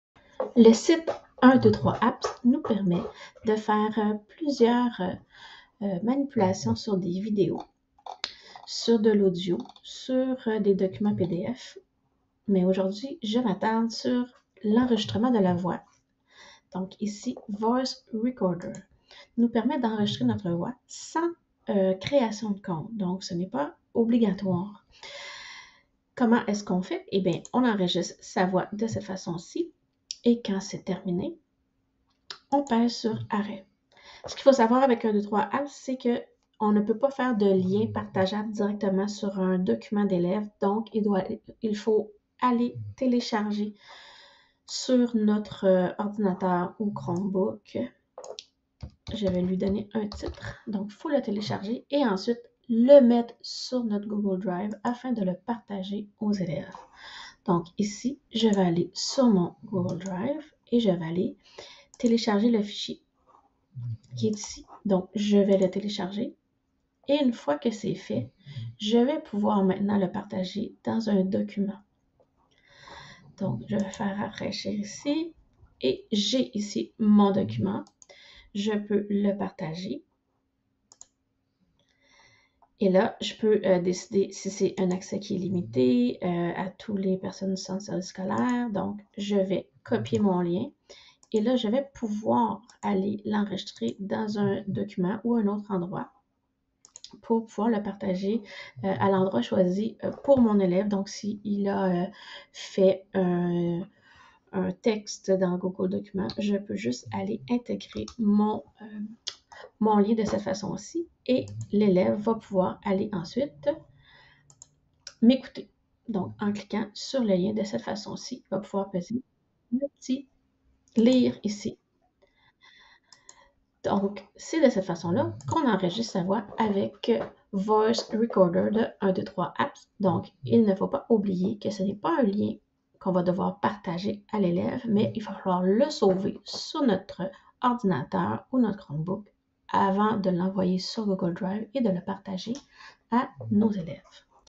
Rétroaction: Voice recorder 123APPS
Comment enregistrer votre voix et partager le document audio dans un document avec Voice recorder de la ressource 123APP.com